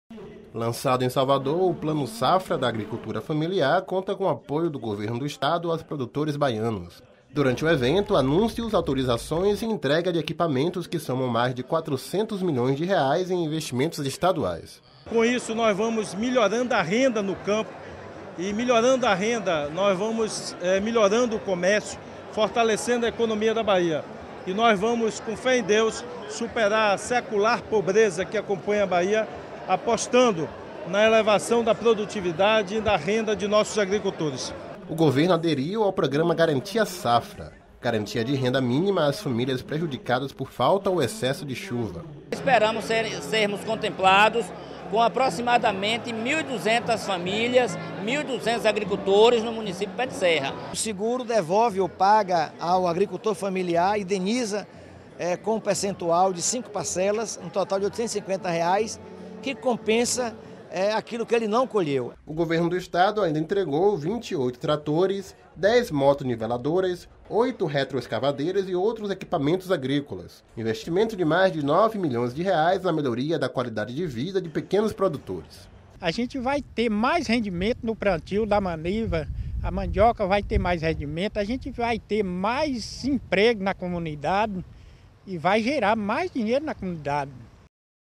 PLANO SAFRA
Plano Safra soma investimentos de R$ 400 milhões para agricultura